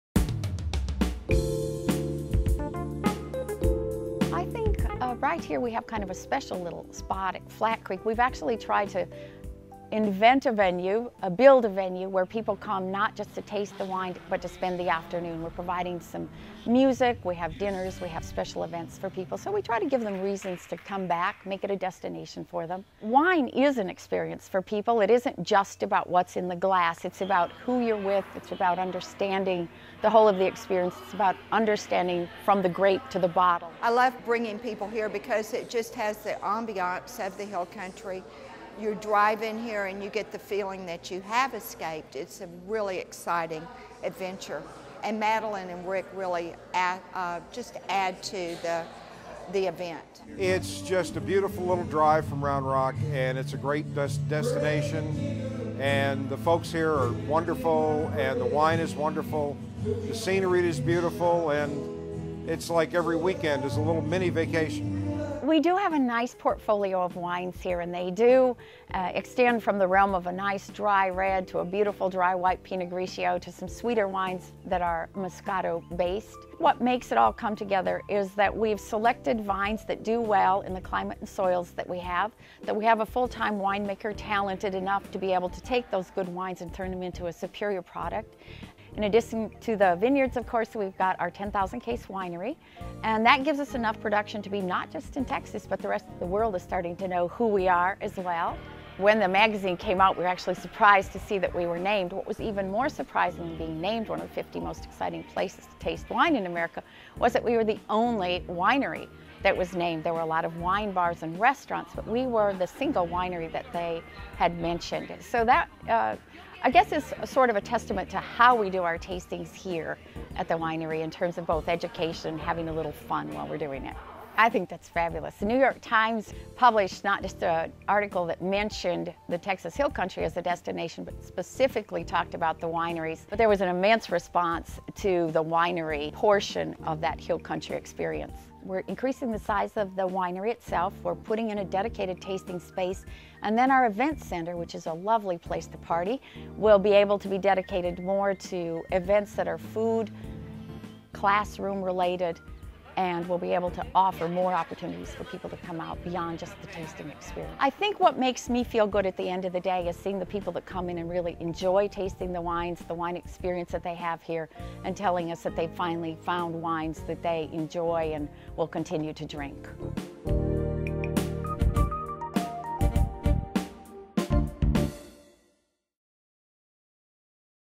Flat Creek Estate Wines
Flat Creek Estate was voted one of the top wine destinations in America according to Food and Wine Magazine. Distributed by Tubemogul.